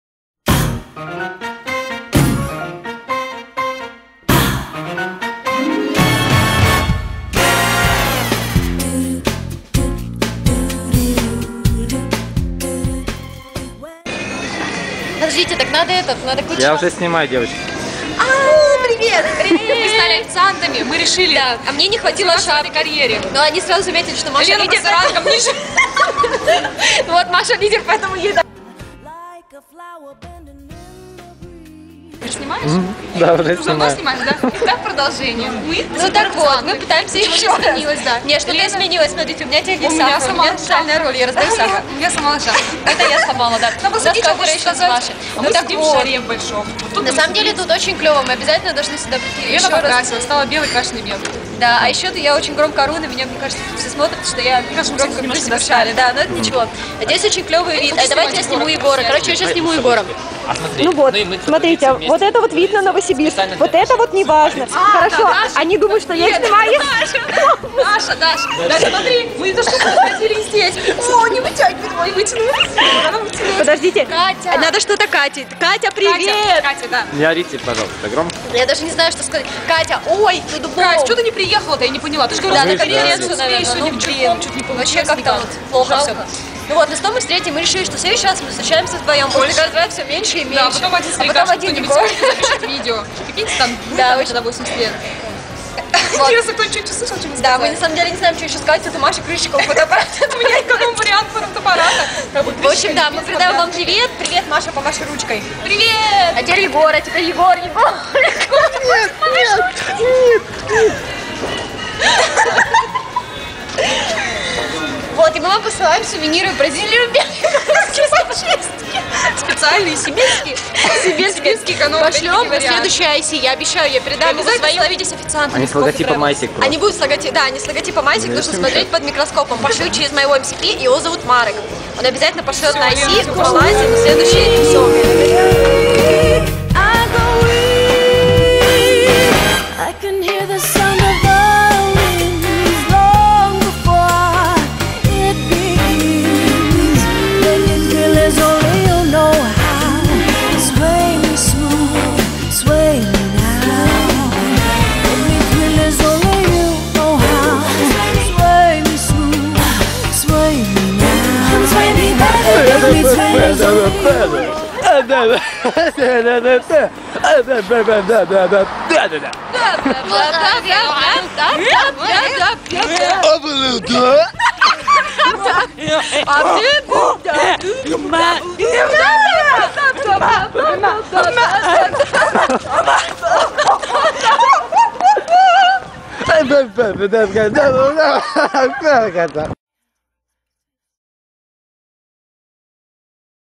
EB nstu 2007-2008
Meeting with LCP, VP TM and VP Com.
Hello to VP OGX (in Brazil) and VP ICX (in Mexico)!

See you in Hugnary! 8)